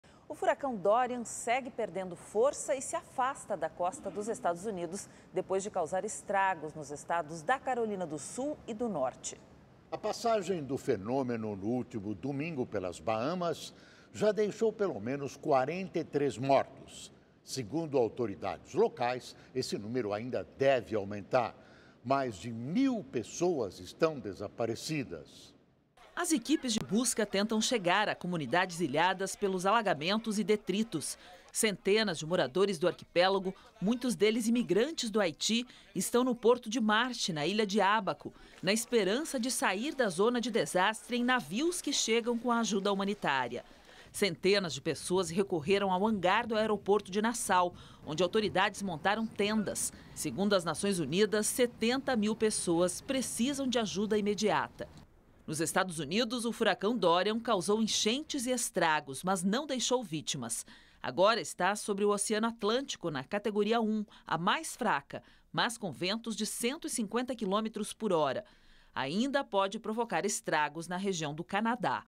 Furacão Dorian deixa 43 mortos e rastro de destruição nas Bahamas
O furacão Dorian segue perdendo força e se afasta da costa dos Estados Unidos, depois de causar estragos nos estados da Carolina do Sul e do Norte. A passagem do fenômeno, no último domingo, pelas Bahamas, já deixou, pelo menos, 43 mortos. Segundo as autoridades locais, esse número ainda deve aumentar. Mais de mil pessoas seguem desaparecidas.